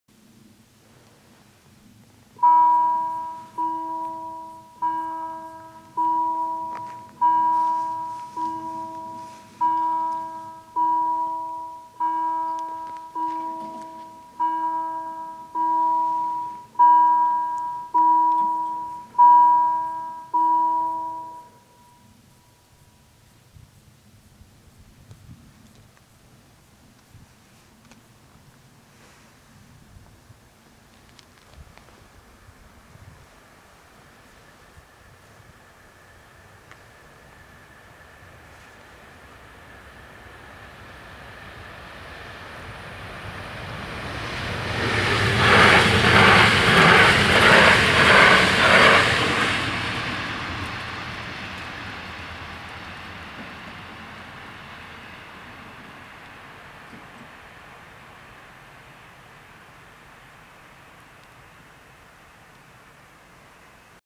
Spoorwegovergang Herzberg (D) // Railroad crossing // Bahnübergang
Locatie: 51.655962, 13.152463
Traject: Falkenberg - Lutherstadt Wittenberg
Ontruimingslichten: 4
Bellen: 2
Bomen: 2
Andreaskruisen: 4
Passeren:
- Talent2 als RB51 Falkenberg → Lutherstadt Wittenberg